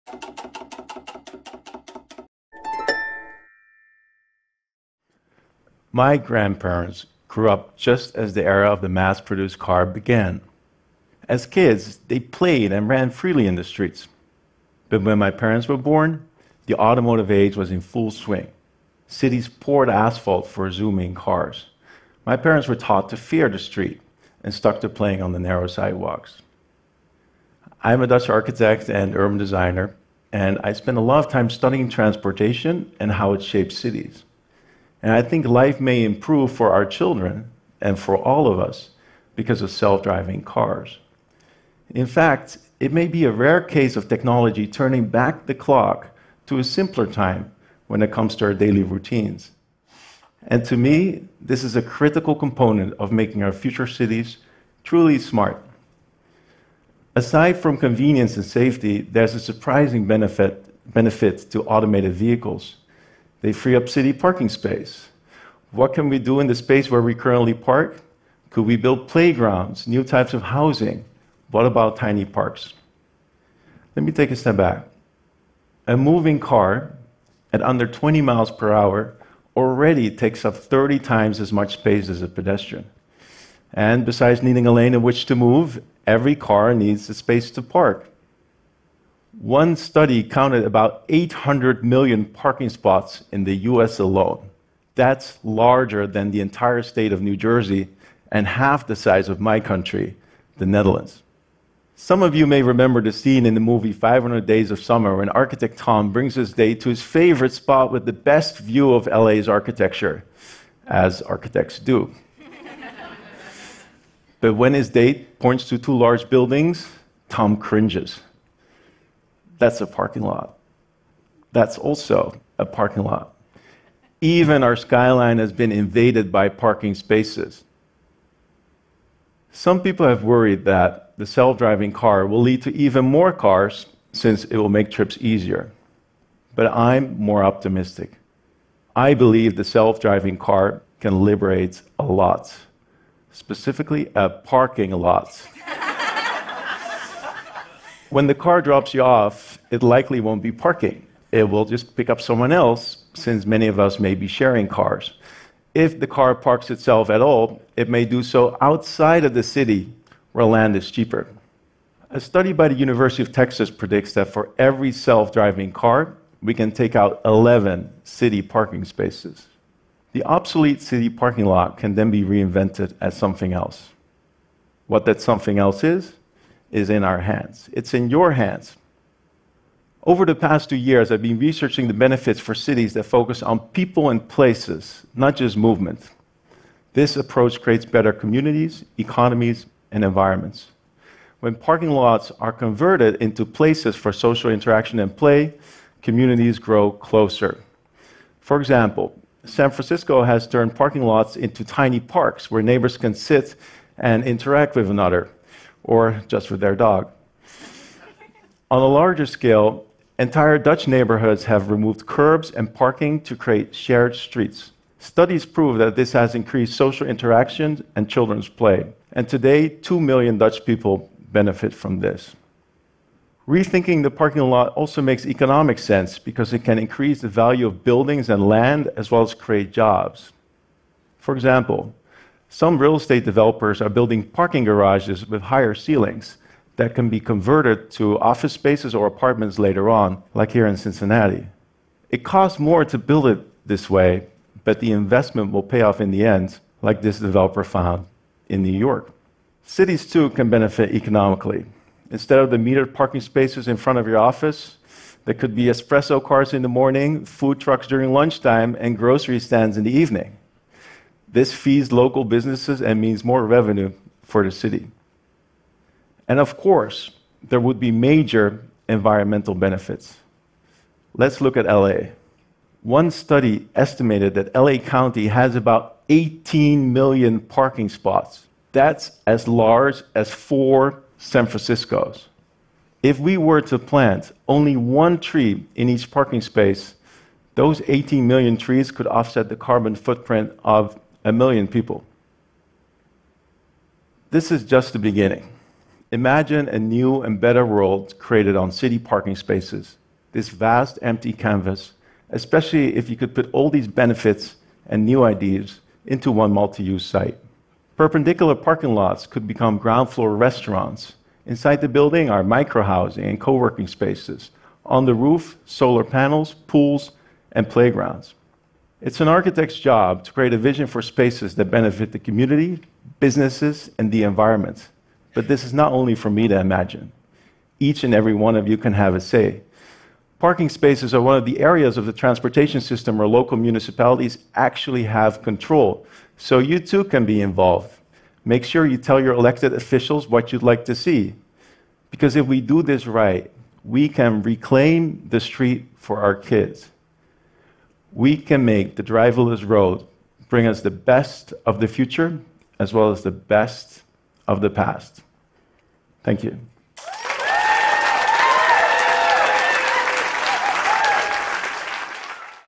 Stefan Al: The future of parking spaces
The age of the self-driving car is here but what happens to all of the parking lots?  Architect Stefan Al suggests for how these newly available spaces might be reimagined and how people can play a role in it.